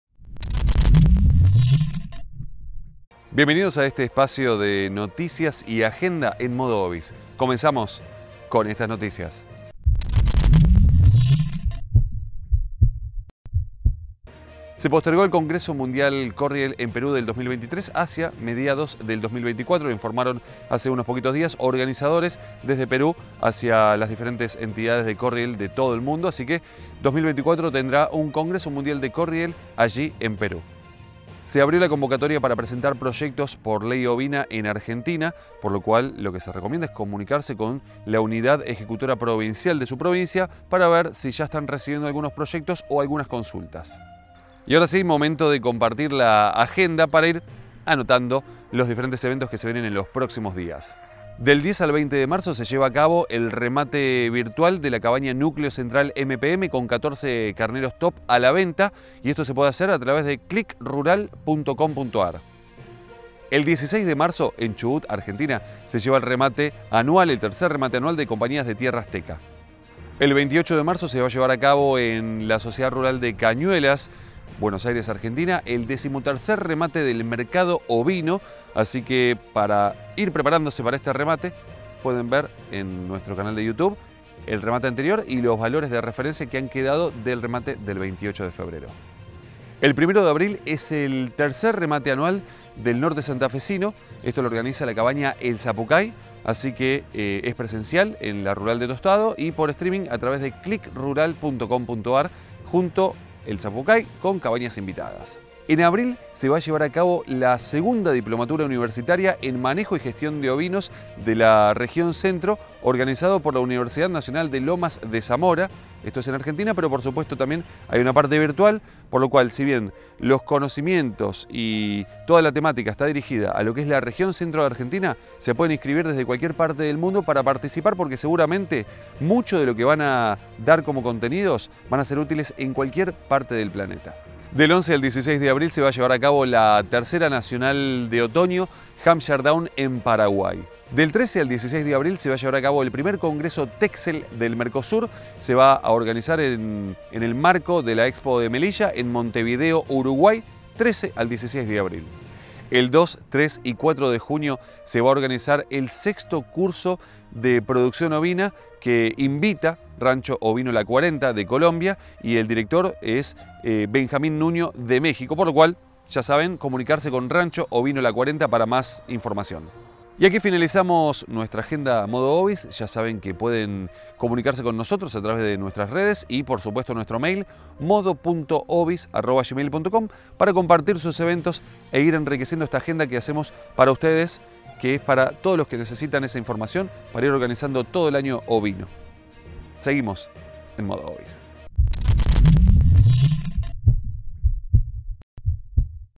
Alerta mundo ovino! Agenda de marzo y abril - NotiOvis
Bienvenidos a la agenda de noticias de Modo Ovis, para que no te pierdas ningún evento del mundo ovino en Argentina y otros países.